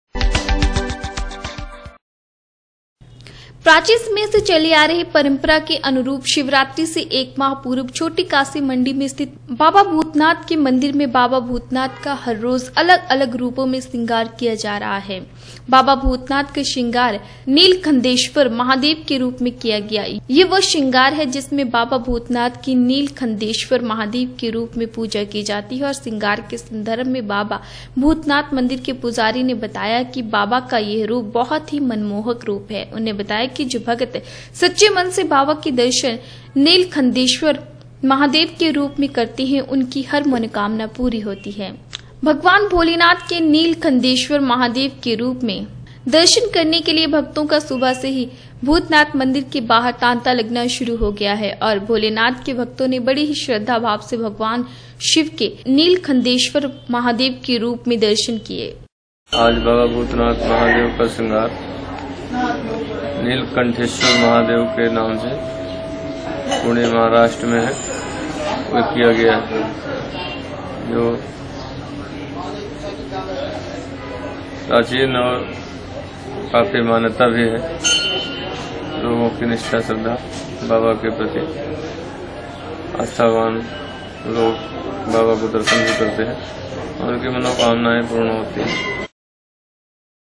बाबा भूतनाथ का सिंगार निलखंथेश्वर महादेव के रूप में किया गया
छोटीकाशी मंडी में स्थित बाबा भूतनाथ का मंदिर वर्तमान समय श्रद्धालुओं के आकर्षण का केंद्र बना हुआ है यहां हर रोज बाबा भूतनाथ का नए नए रूपों में सिंगार किया जाता है इसी कड़ी में बाबा भूतनाथ का सिंगार निलखंथेश्वर महादेव के रूप में किया गया